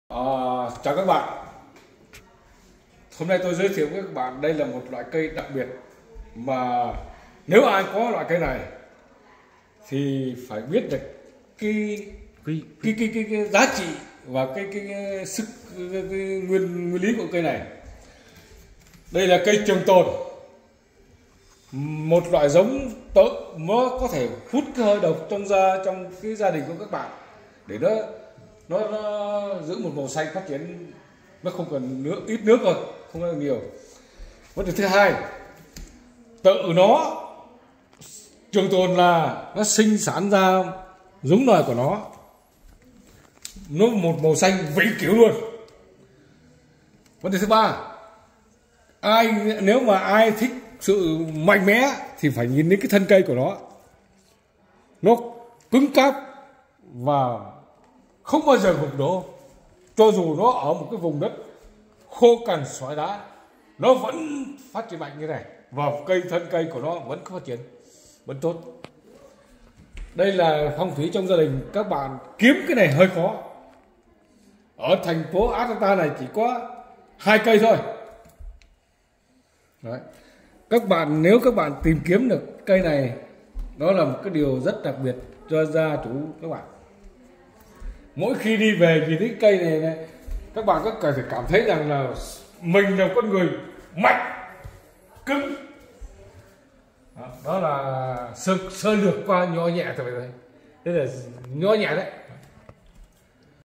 chuyên gia phong thủy nói về cây trường sinh bất lão
PHUONG YEU ATLANTA